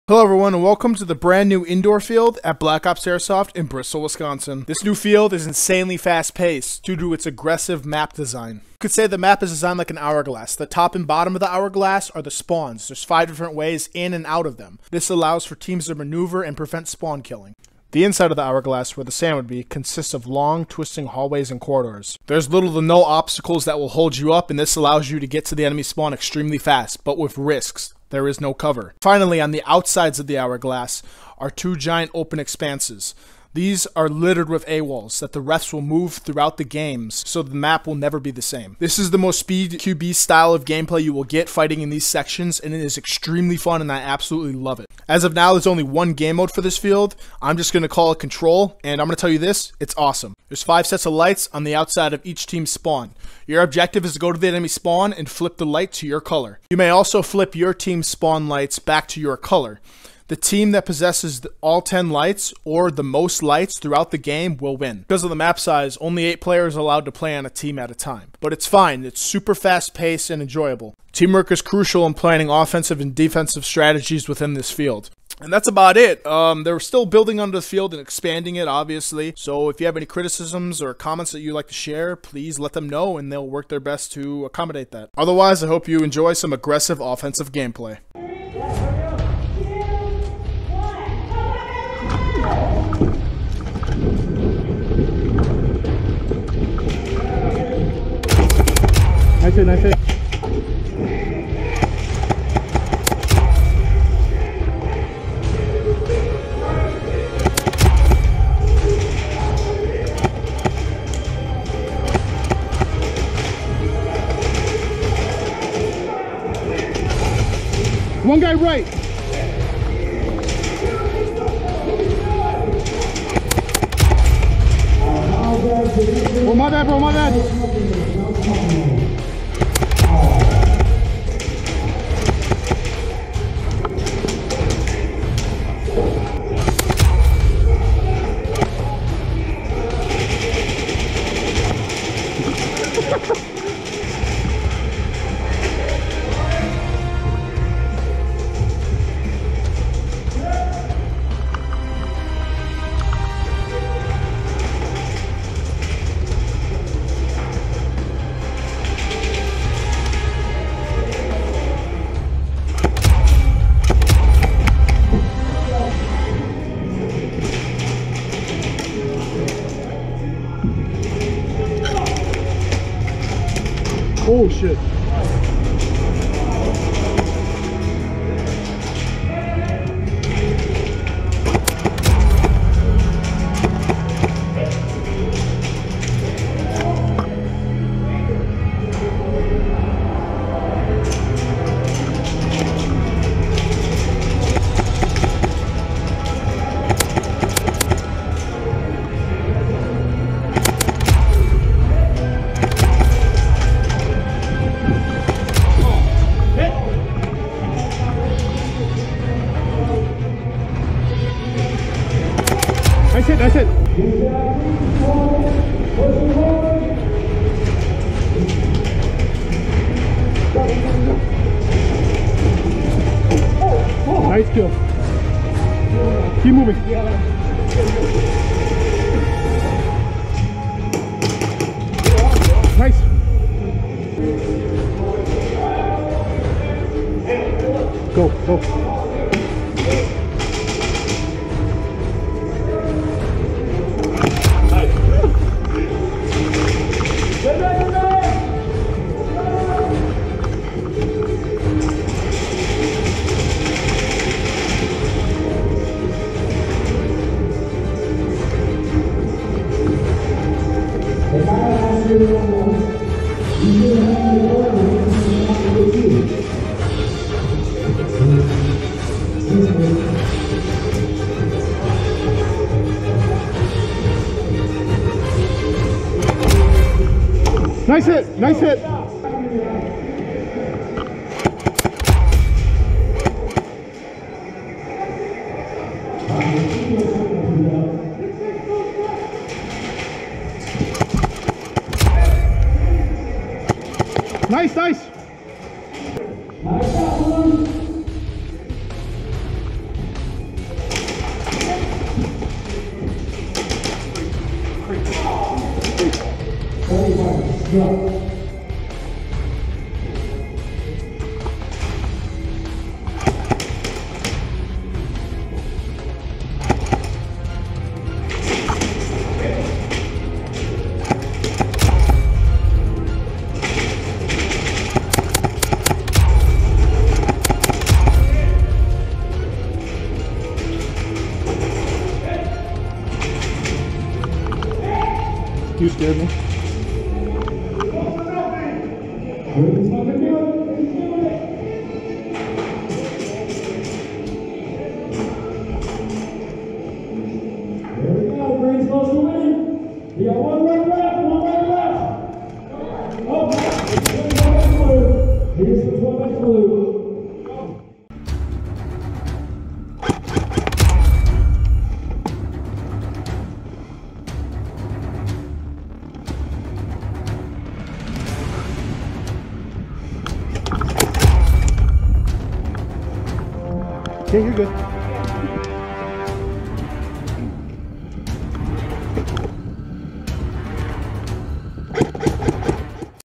NEW BOA INDOOR Field Gameplay! | Black Ops Bristol, WI
⏰Timestamps ⏰
0:04​​​ Intro
1:28 Info About Field
1:40 Go Time
2:23 Momentum
2:45 So Close
3:33 Score One
3:54 Gain Ground
4:38 Hard OBJ
4:56 Cut Short
4:55 Repel
5:58 Ending

What do you think about the new addition to BOA if you played already?